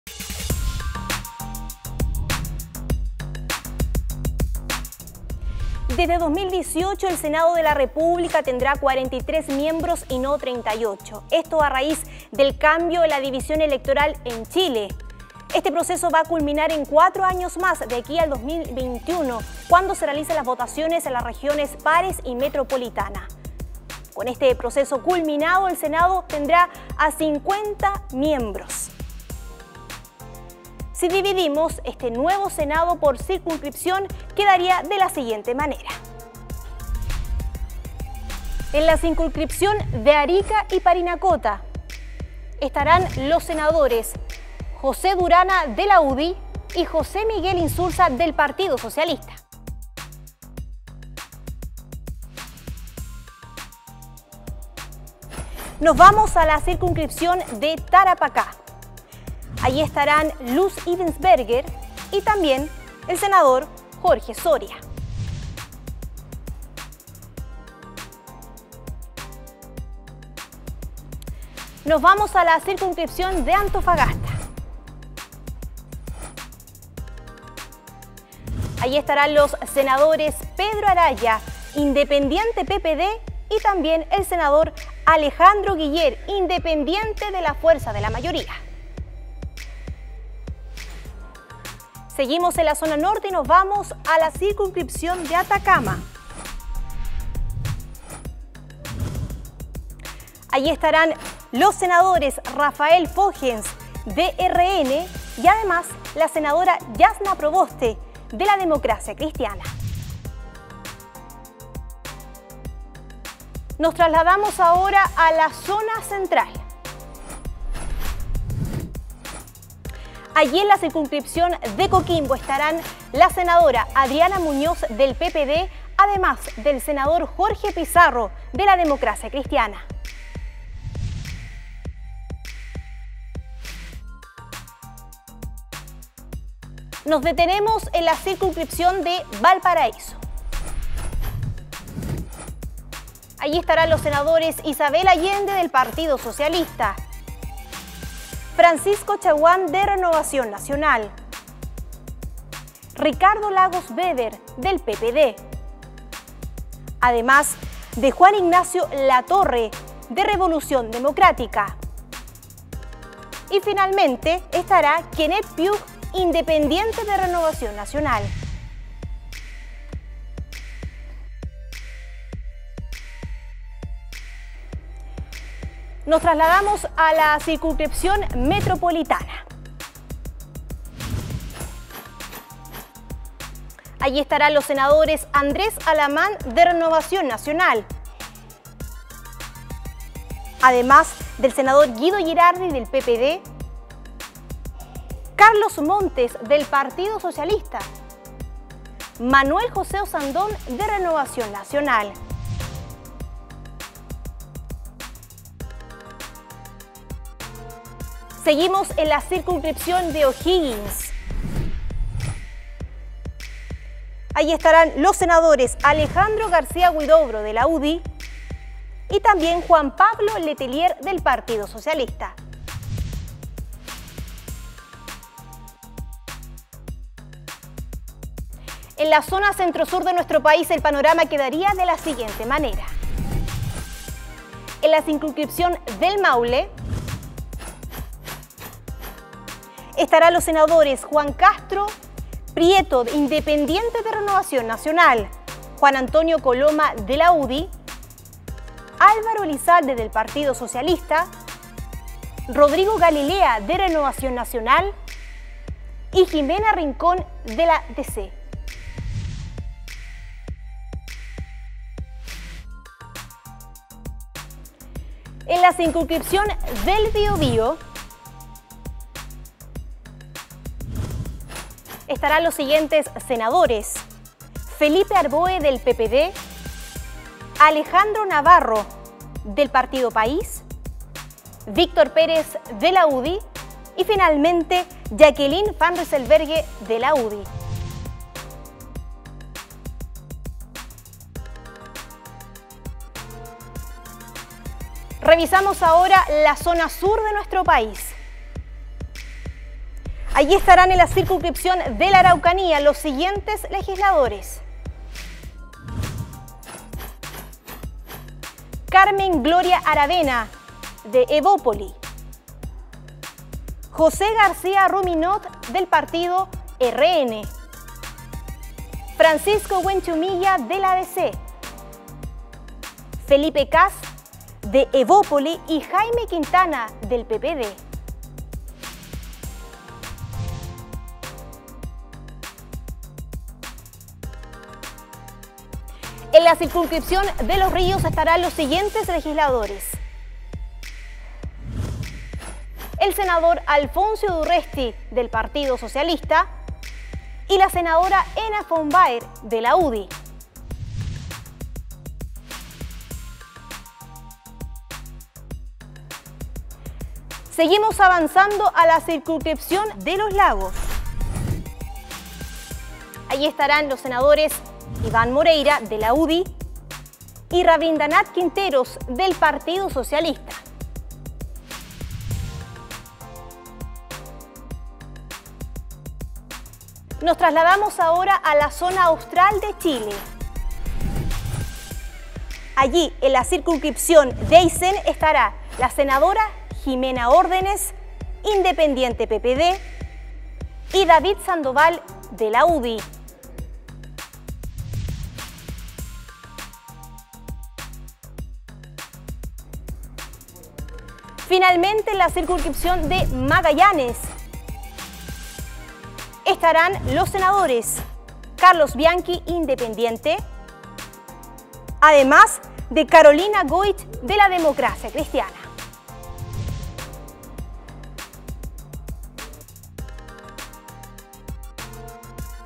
EL NUEVO SENADO - 2018 - 2022
Nueva Legislatura: se declara solemnemente instalado el Senado
Con el juramento y/o promesa de los nuevos legisladores y la elección de la mesa compuesta por el senador Carlos Montes, presidente y el senador Carlos Bianchi, vicepresidente se realizó la sesión de instalación de esta legislatura, donde además debuta la nueva composición del Parlamento.